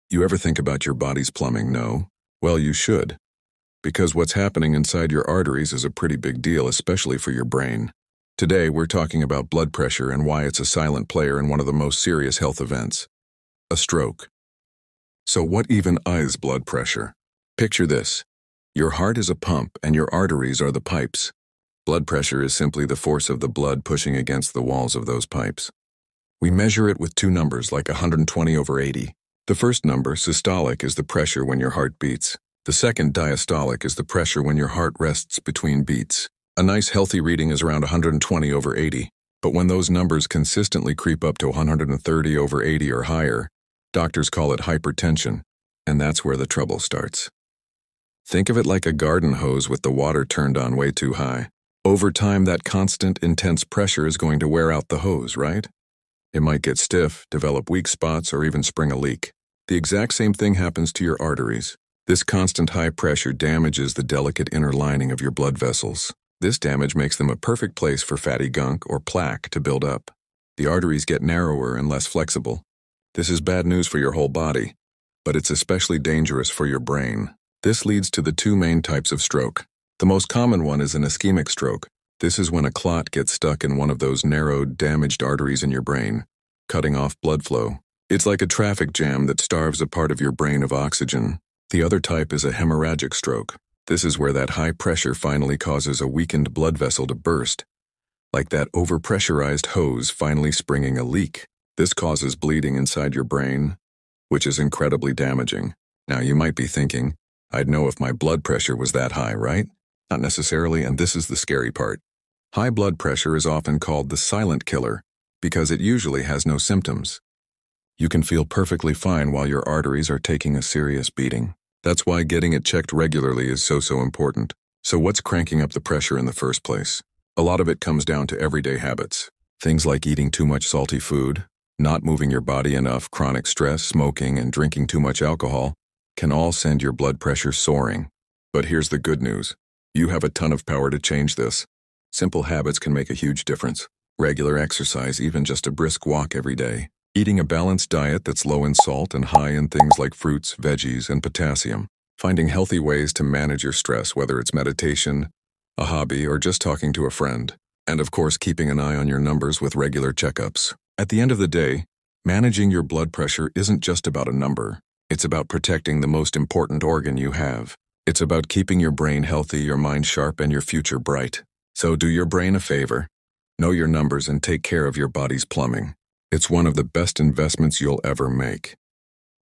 "Why High Blood Pressure Is a Brain Emergency (Not Just a Heart Problem)
High Blood Pressure & Stroke Risk Explained | Science You Can Feel

Did you know high blood pressure is the #1 risk factor for stroke—and most people don’t even feel it coming? In this video, we break down the science behind hypertension and how it silently damages your brain’s blood vessels.

You’ll learn:
✅ What blood pressure really means
✅ How it leads to stroke (with real-life analogies)
✅ Daily habits that raise or reduce your risk
✅ Simple ways to protect your brain and heart

Whether you're curious, concerned, or just love science that makes sense—this one's for you.

StrokePrevention